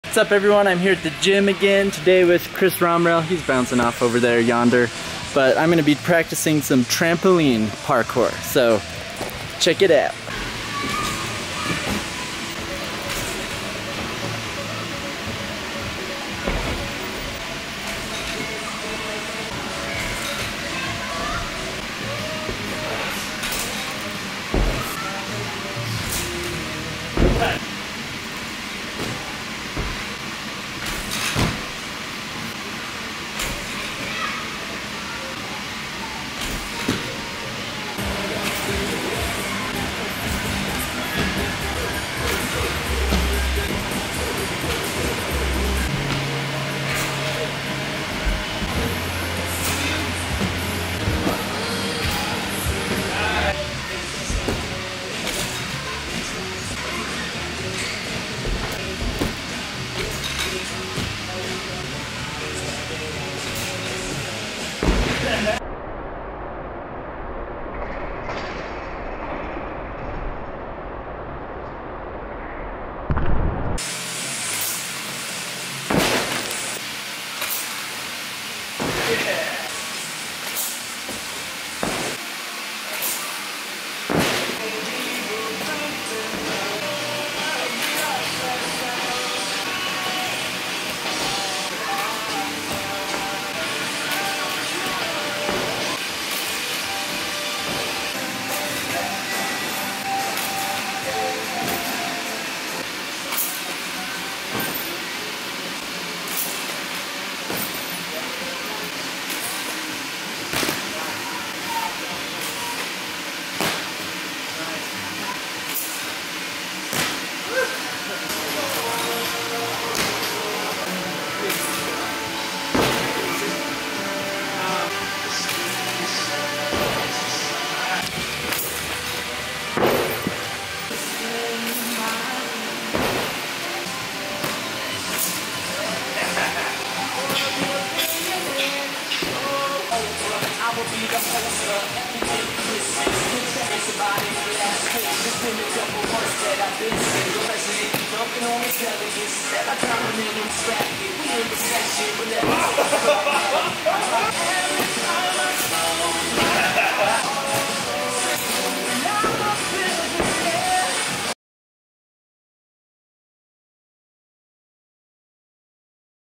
Mini Tramp Parkour Training
Decided to try something new and train some various parkour movements with the aid of a mini tramp.